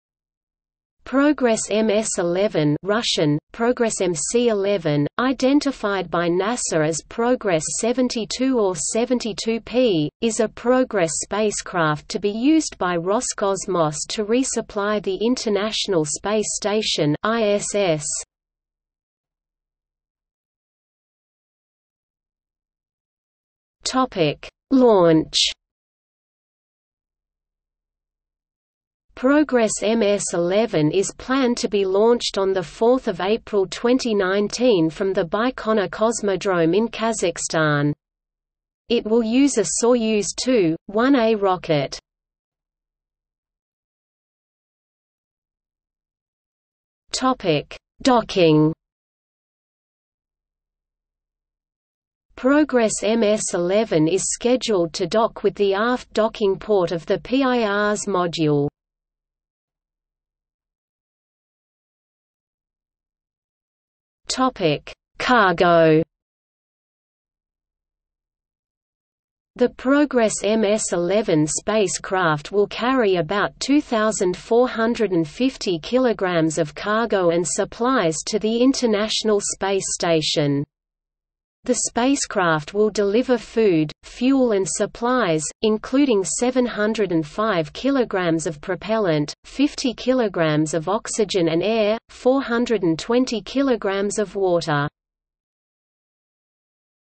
Progress MS-11 | Wikipedia audio article
00:00:20 1 Launch
00:00:43 2 Docking
00:01:00 3 Cargo

- Socrates

SUMMARY
Progress MS-11 (Russian: Прогресс МC-11), identified by NASA as Progress 72 or 72P, is a Progress spacecraft to be used by Roscosmos to resupply the International Space Station (ISS).